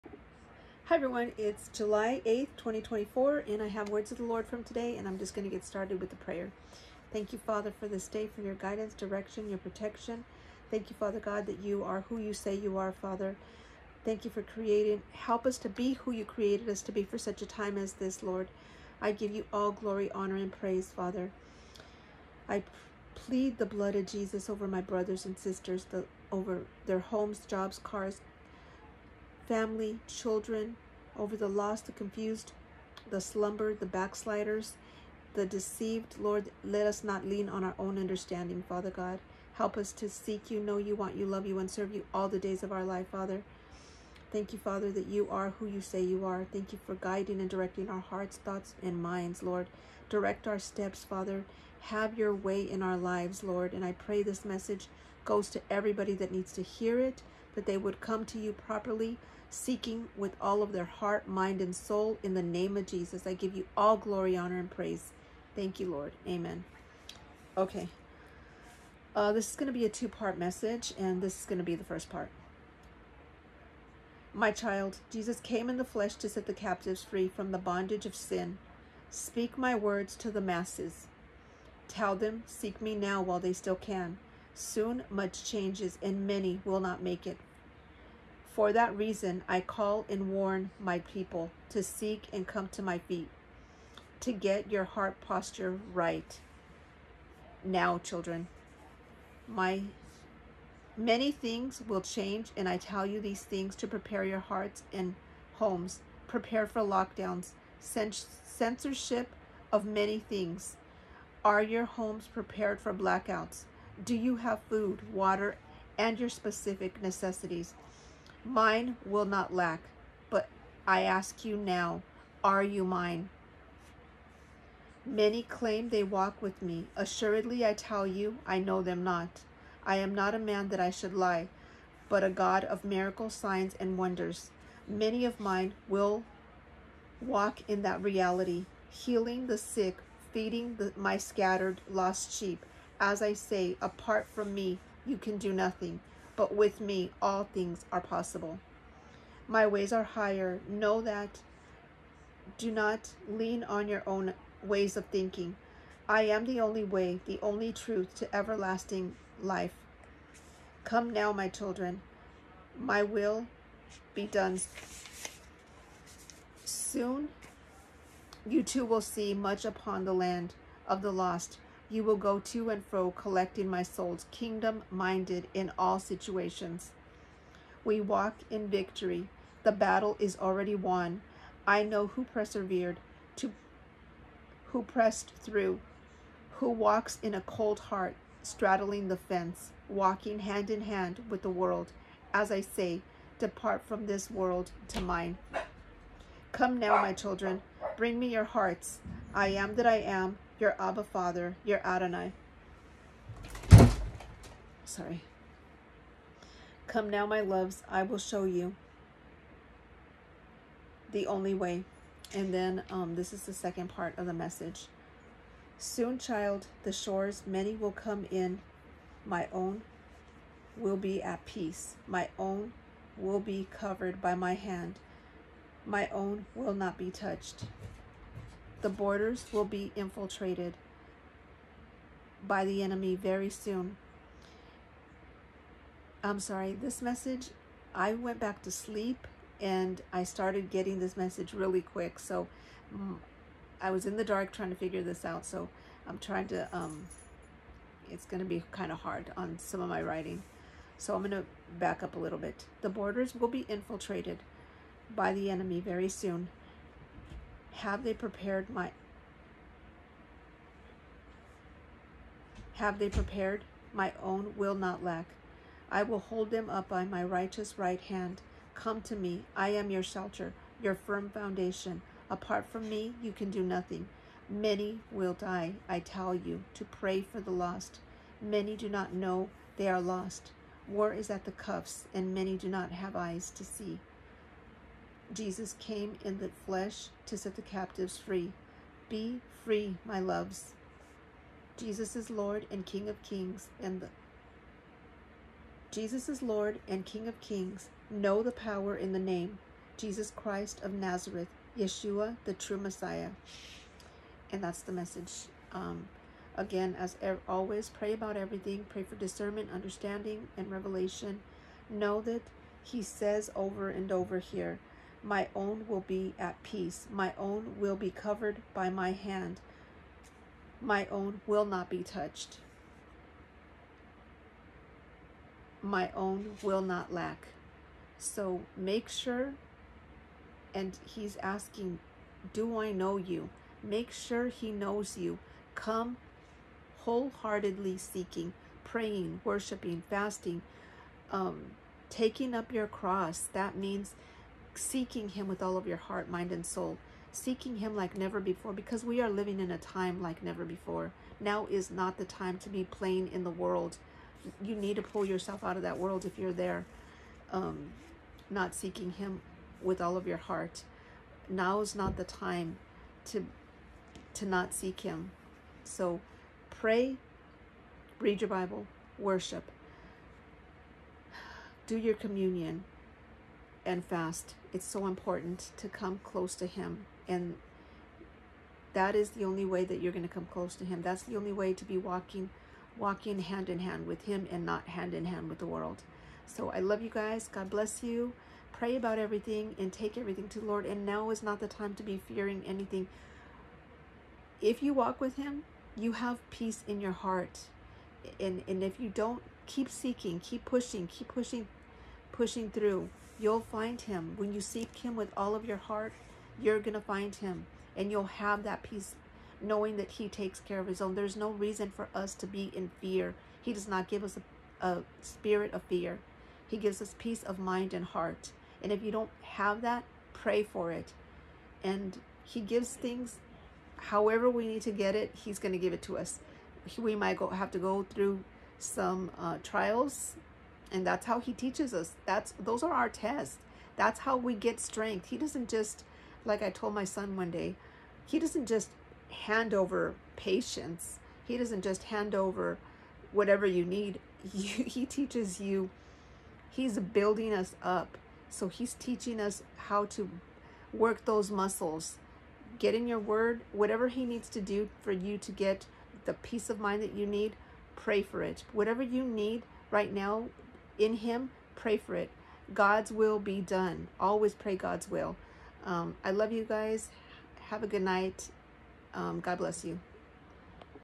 War is at the cuff and many do not have eyes to see. Come now, my loves I will show you the only way
Prophetic words of the Lord thought, Almighty and Yeshua Hamashiach the true Messiah
1 John 4:3
and every spirit that does not confess that Jesus Christ has come in the flesh is not of God. And this is the spirit of the Antichrist, which you have heard was coming, and is now already in the world.

Isaiah 55:6
Seek the Lord while He may be found,
Call upon Him while He is near.

Proverbs 4:23
Keep your heart with all diligence,
For out of it spring the issues of life.

Numbers 23:19
“God is not a man, that He should lie,
Nor a son of man, that He should repent.
Has He said, and will He not do?
Or has He spoken, and will He not make it good?

Acts 2:22
“Men of Israel, hear these words: Jesus of Nazareth, a Man attested by God to you by miracles, wonders, and signs which God did through Him in your midst, as you yourselves also know

Ezekiel 34:5-6
Scattered sheep

John 15:5
“I am the vine, you are the branches. He who abides in Me, and I in him, bears much fruit; for without Me you can do nothing.

Matthew 19:26
But Jesus looked at them and said to them, “With men this is impossible, but with God all things are possible.”

Proverbs 3:5
Trust in the Lord with all your heart and lean not on your own understanding;

John 14:6
Jesus said to him, “I am the way, the truth, and the life. No one comes to the Fatherexcept through Me.

1 Corinthians 15:57
But thanks be to God, who gives us the victory through our Lord Jesus Christ.

Isaiah 41:10
Fear not, for I am with you;
Be not dismayed, for I am your God.
I will strengthen you,
Yes, I will help you,
I will uphold you with My righteous right hand.

Matthew 7:24-27
Thereforewhoever hears these sayings of Mine, and does them, I will liken him to a wise man who built his house on the rock: and the rain descended, the floods came, and the winds blew and beat on that house; and it did not fall, for it was founded on the rock. “But everyone who hears these sayings of Mine, and does not do them, will be like a foolish man who built his house on the sand: and the rain descended, the floods came, and the winds blew and beat on that house; and it fell. And great was its fall.”